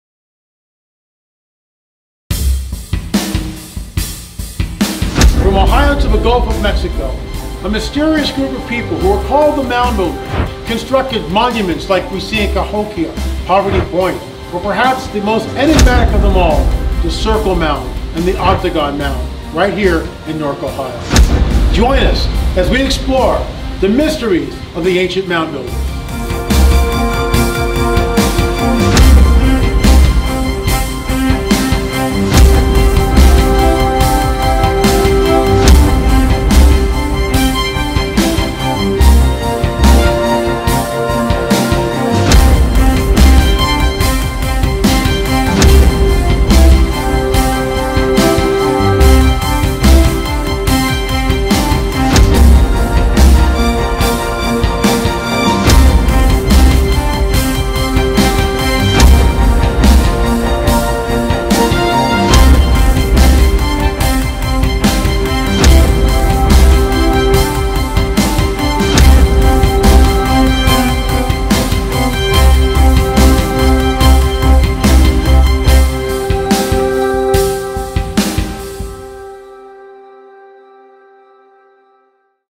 Mathematical Mysteries of the Moundbuilders! Episode #2
Two millennia ago a culture that is still unknown to us today erected thousands of mounds throughout the midwestern United States to the Gulf of Mexico.
 They are called the Hopewell, the Adena, or the Misssissippian… however, no one knows what these people actually called themselves. In other words they remain mysterious and unknown us today.

 When the first white settlers came into The Great Circle Mound in Ohio they asked the Native Americans living there… who built this. The Natives replied that they didn’t know who built them as these large mounds were there and abandoned when they arrived.

 The discovery of these mounds created a controversy as some posited the mounds were built by an unknown lost tribe of people, who migrated, perhaps, from the old world.
 Others thought they could be one of the lost tribes of Israel.
 This debate continued until the late part of the 19th century when the conclusion was made that Native Americans had built the mounds but they had simply forgotten  they had done so.

 This is now the accepted paradigm. However, is it the truth of what occurred in the ancient past or has the information been managed to bolster a particular narrative?

 Join us as we explore the Mysterious Moundbuilders…